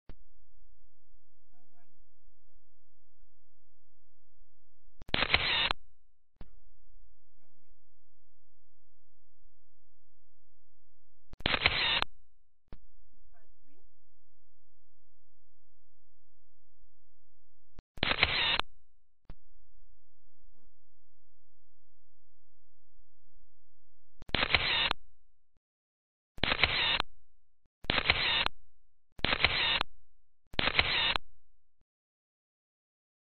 HardRock photo boothing with Monique
Check out my video from a Hard Rock Cafe photo booth.

Spending a lovely morning at Hard Rock Cafe' Dubai having lunch with Monique. Some UAE fun before she is off to Kuwait to start teaching. Love you lots my baby.